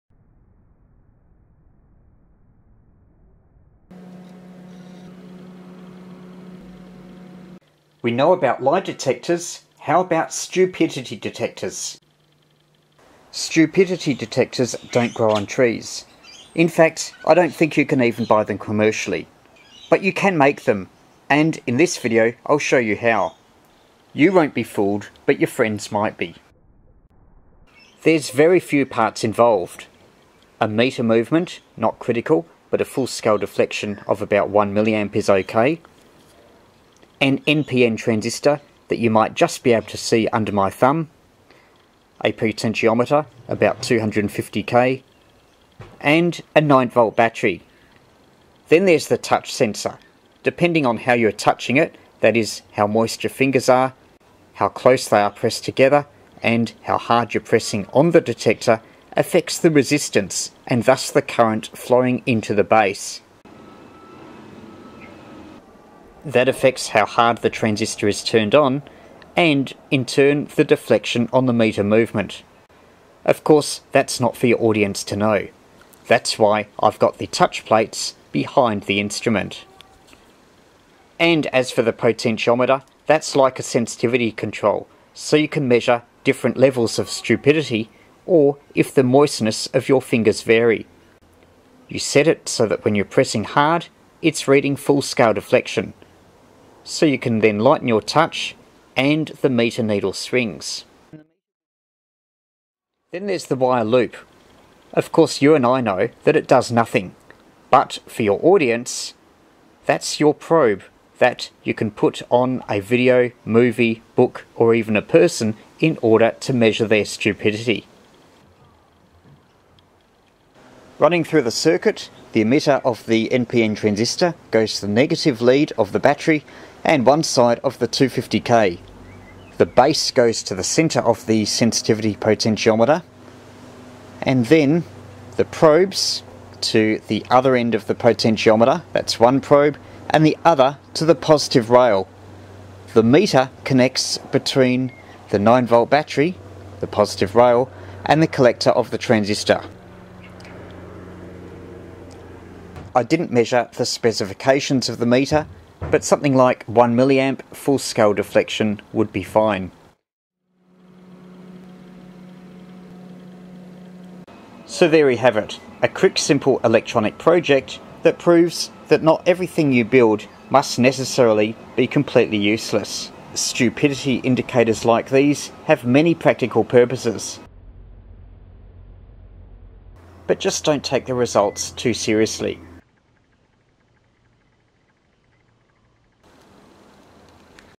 Electronics on the Floor: Build an electronic stupidity meter
A project to intrigue those not into electronics.

Parts required:

* Analogue meter movement (0-1mA approx)
* NPN transistor (eg BC548, 2N3904, 2N2222 etc)
* 250k variable resistor
* printed circuit board material for touch pads
* 9 volt battery